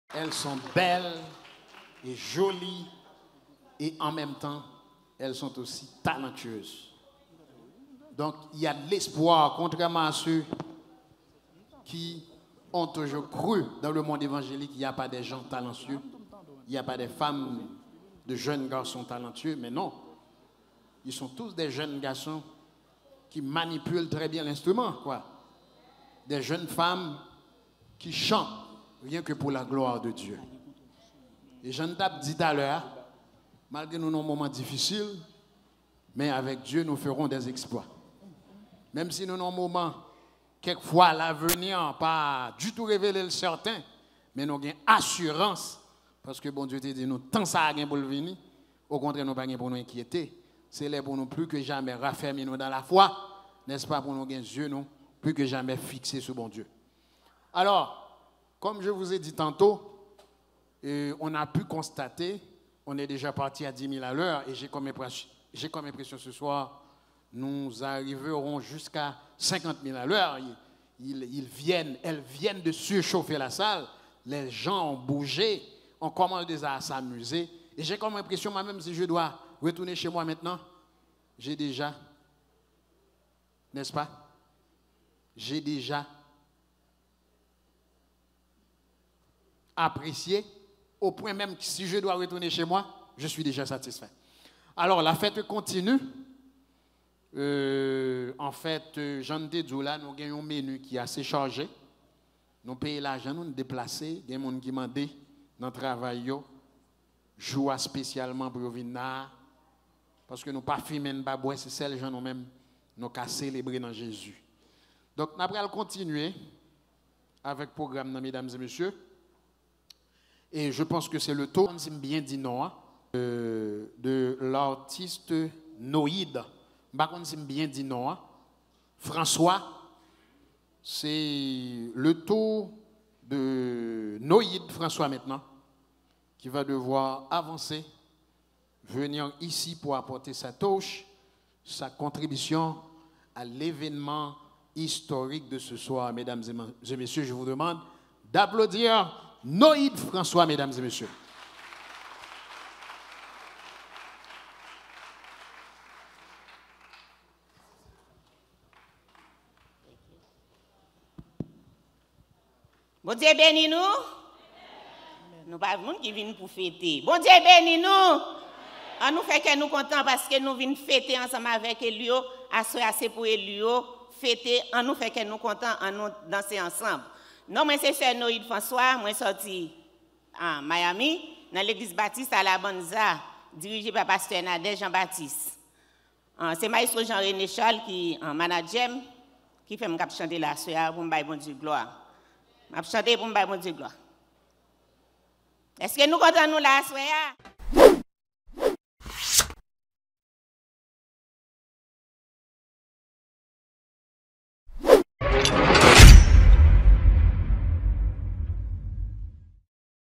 Host Of The Night Jean Eddy Ulysse | Pral Prezante Nou Pwochen Atis La | Noide Francois
EER ElioEnai Presents 5th Album Release Fundrading Concert
March 11, 2024
Playlists Of The Night
Host: JeanEddyUlysse And Marie Solette Jerome
Artist: Elio Enai Rapha -
Naromie Buissereth - Fritzel Adrien Saxophone -
 - Jean Rene Charles - Noide Francois - Joline Romelus